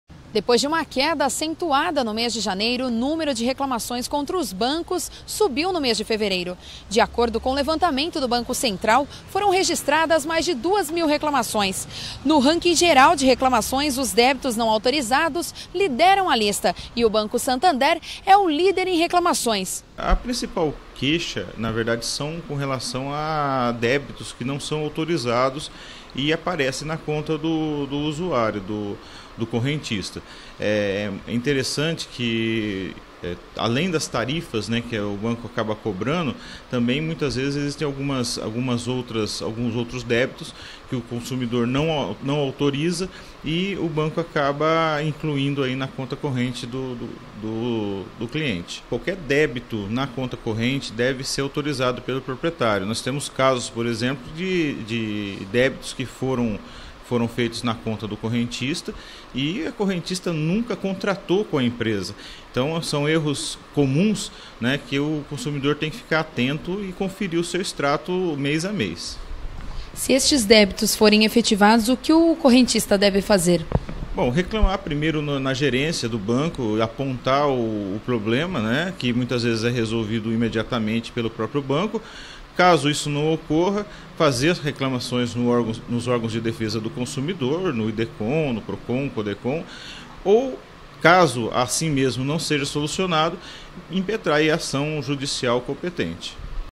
TV Ara - Reclamações contra bancos aumentam em fevereiro
Uma média de 1.777 reclamações foram registradas no mês de fevereiro em relação aos bancos. O número aumentou comparado ao mês de janeiro.